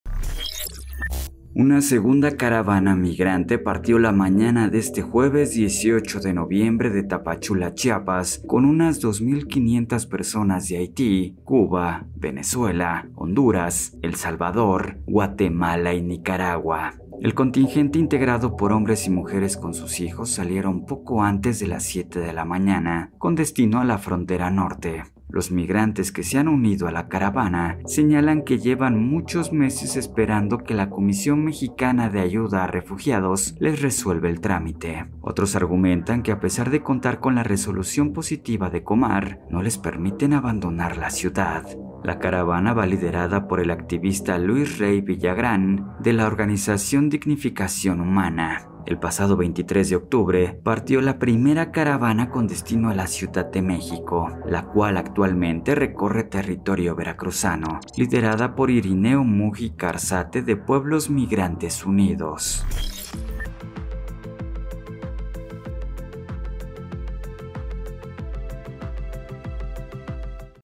Sale segunda caravana migrante desde Tapachula
El contingente integrado por unas 2 mil 500 personas salieron poco antes de las 7:00 de la mañana con destino a la frontera Norte